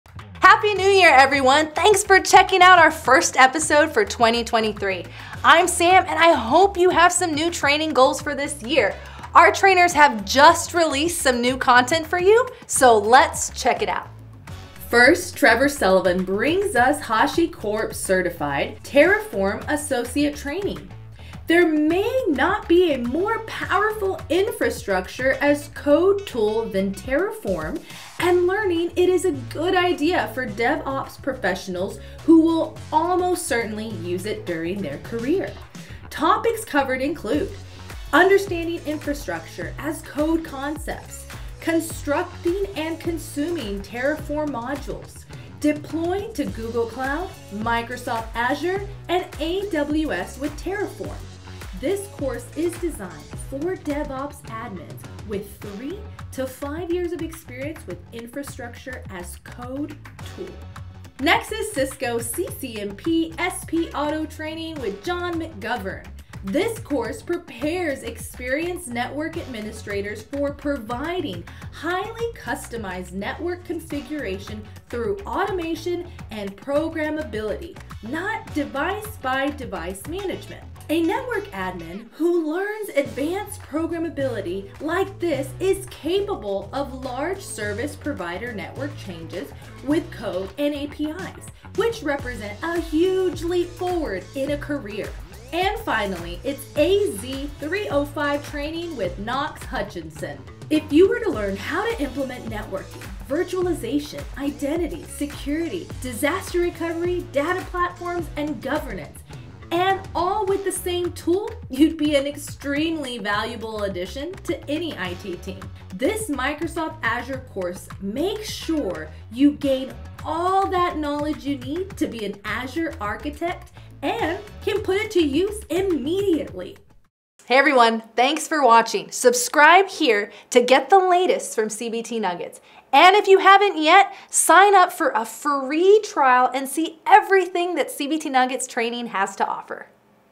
New IT Training Courses | CBT Nuggets
Our trainers have been busy. In the past month, we’ve released a wide range of training designed to help IT pros like you achieve your goals in 2023. Learn about some of our newest courses, which cover everything from Cisco networking to implementing Microsoft Azure solutions to coding tools like Terraform!

We're hiring trainers!